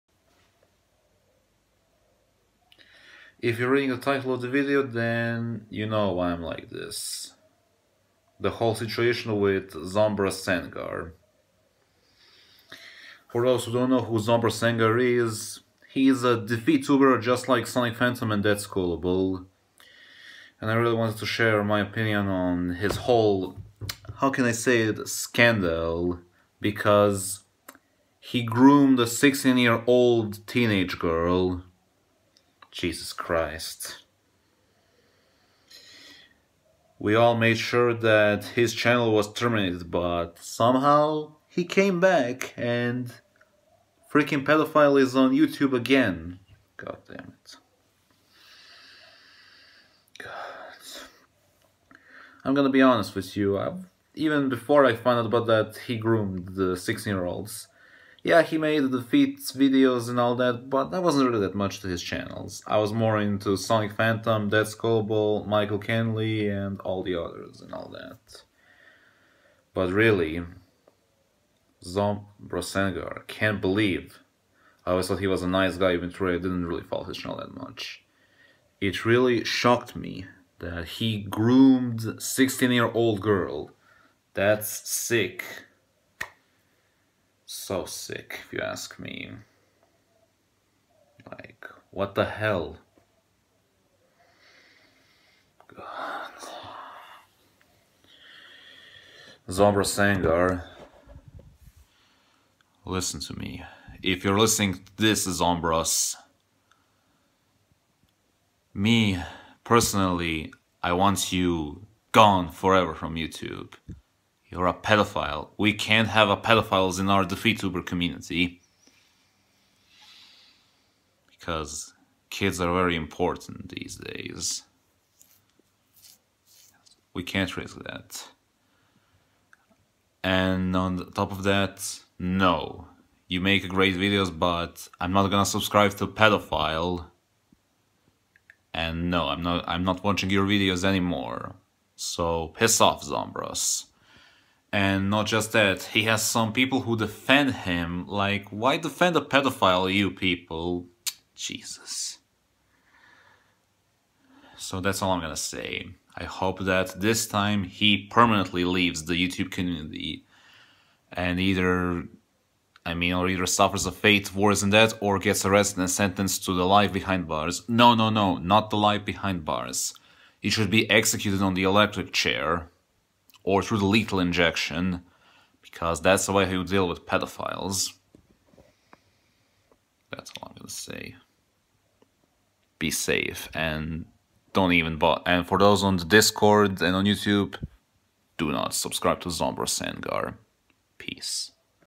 My thoughts on situation on Zomboss + warning message to Zombross
Well i was gonna share my toughts ever since incident with Mr Pedophile and also my very 1st video of 2023(this is also shoutout to Sonicphantom, Somber and Deadskullable) hopefully kids on internet stays away from likes of Zombross